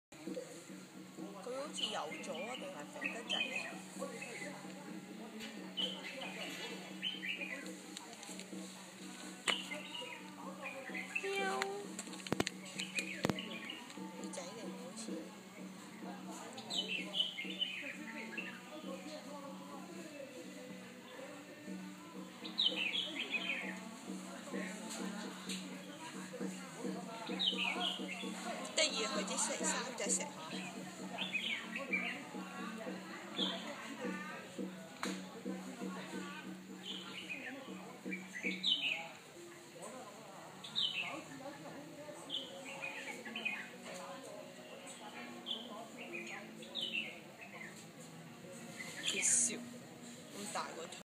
花貓愛自潔^^
花貓躺在石䃈上一邊曬太陽一邊整理毛髮，不顧路人眼光！